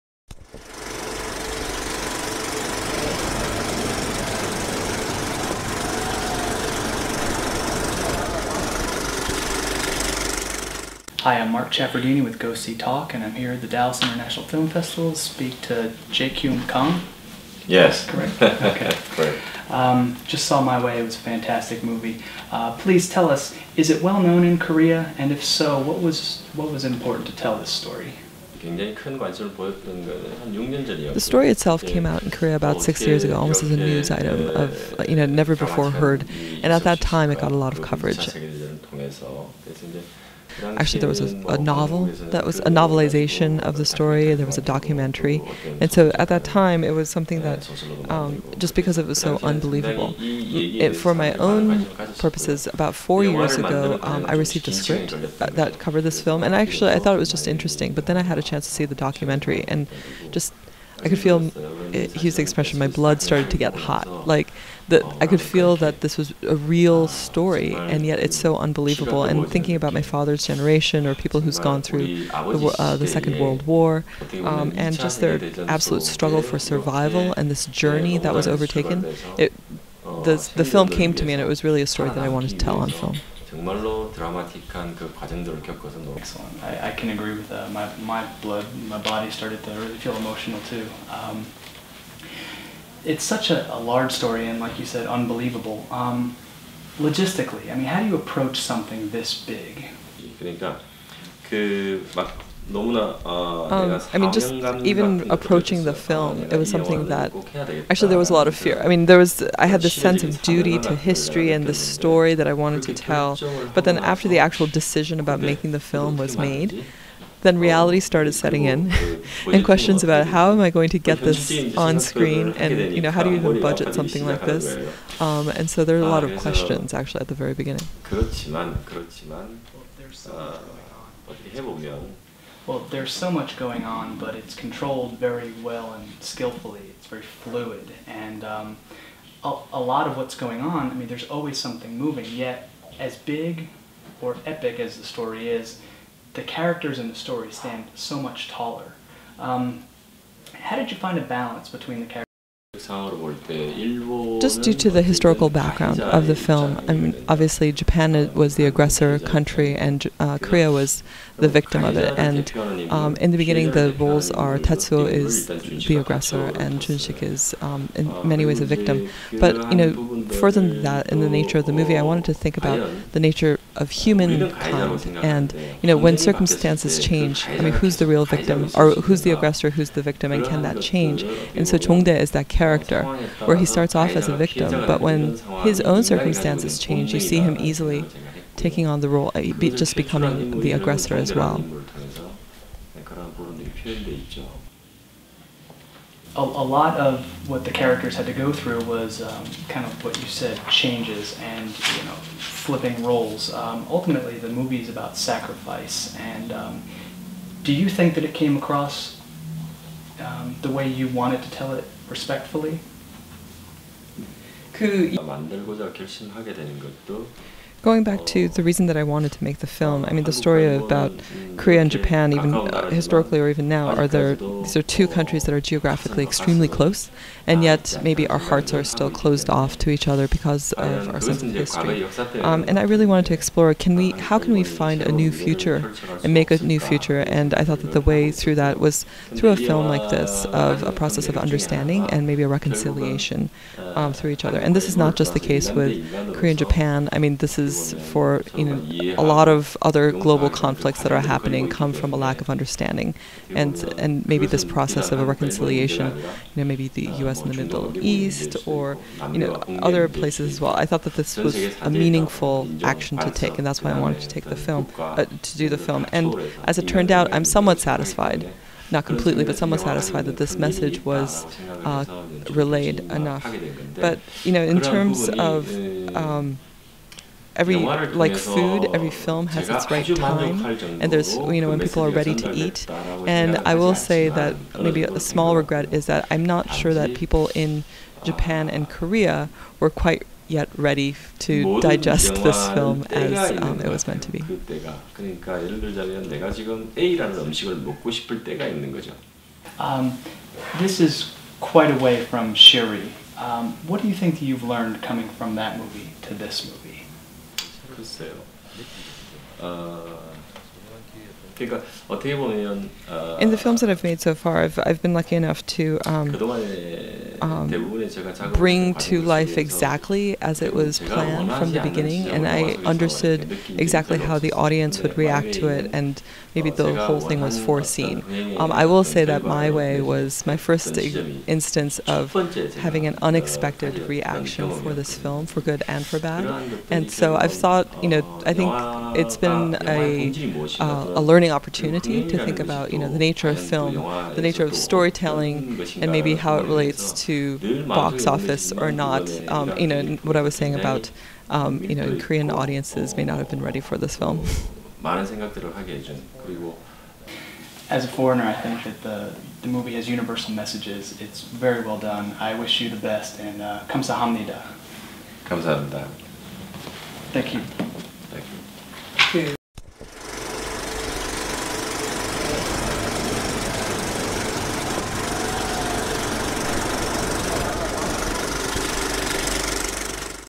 MY WAY Interview - Je-kyu Kang
We got the chance to sit down at the 2012 Dallas International Film Fest for in interview Korean director Je-kyu Kang to talk about his latest film 'My Way'.

About this film:
Unbelievable, True Story, and Epic are but a few terms used to describe this absolutely amazing film from Korean director Je-kyu Kang. MY WAY tells the story of two childhood friends whose lives are drastically changed when they are both forced to enlist in the Army. WWII serves as the backdrop for an almost Shakespearean story about friendship, loyalty, pride and more. The only thing more amazing than the scenes and sequences in the story is the fact that it's based on true events that have only recently come to light in the last 6 years.